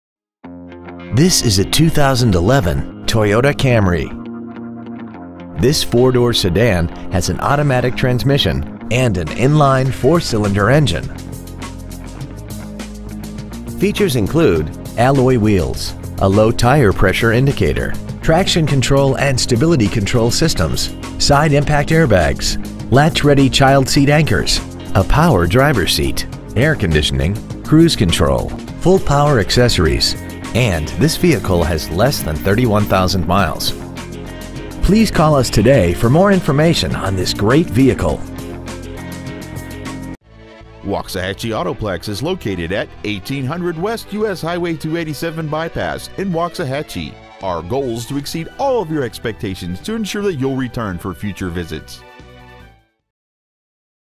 2011 Toyota Camry Corsicana TX
Year : 2011
 Make : Toyota
 Model : Camry
 Engine : 4-Cyl 2.5 Liter
 Trans . : 6-Spd Automatic
 Exterior : Gray
 Miles : 30,113
 Interior : Gray
 Stock : F228856A

 1800 W. US Highway 287 Bypass

 2011 Toyota Camry Waxahachie TX